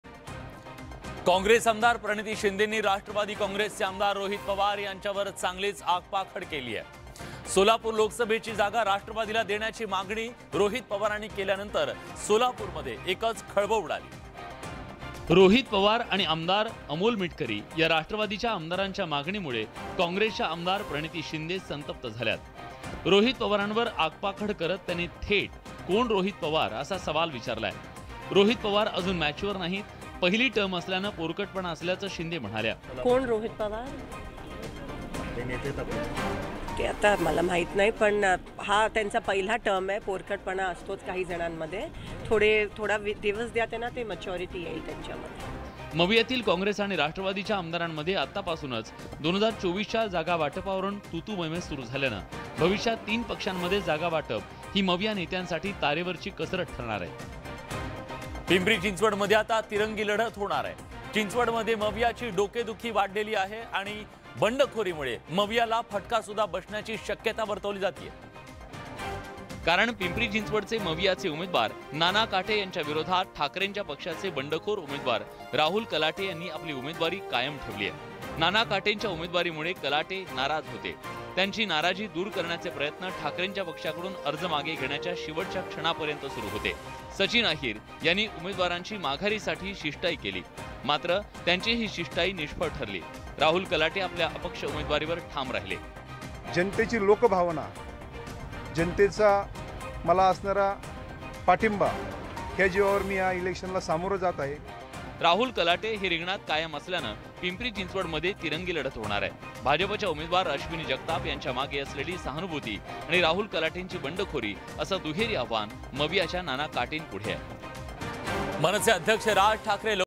MLA Praniti Shinde: सोलापूर मतदारसंघावरुन प्रणिती शिंदे आणि रोहित पवार यांच्यात जुंपली | News 18
There is a war of words between MLA Praniti Shinde and Rohit Pawar from Solapur constituency. Praniti Shinde has severely criticized Rohit Pawar.

सोलापूर मतदारसंघावरुन आमदार प्रणिती शिंदे आणि रोहित पवार यांच्यात वाकयुद्ध रंगलं आहे. प्रणिती शिंदे यांनी रोहित पवारांवर सडकून टीका केली आहे.

n18oc_news